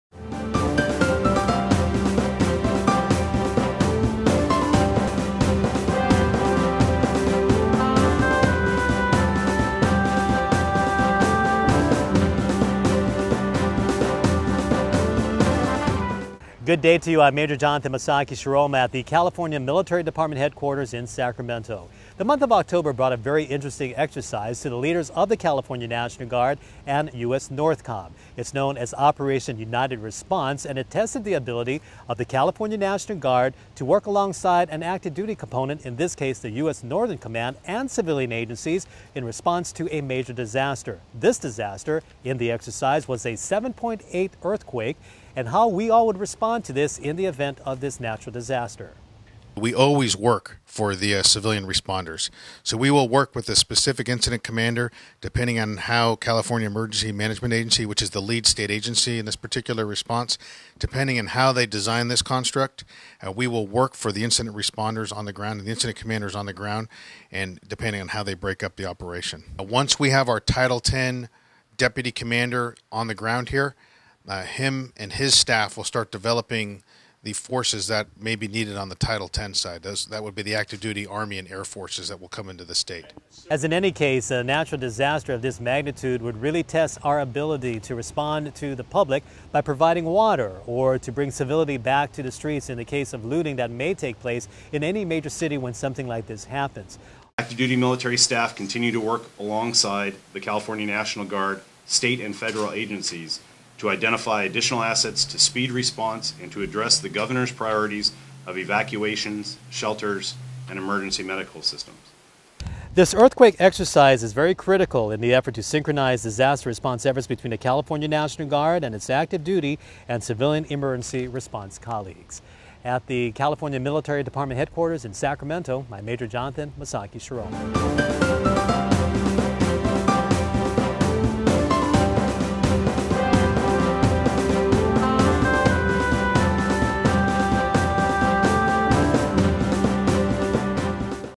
Dual Status Commander Exercise
This video is our own news package called GrizCast and it shows the first ever Dual Status Commander exercise of its kind in the nation. DSC is a construct developed from lessons learned from our military response during Hurricane Katrina. Shot and edited by Tech. Sgt. Prouse.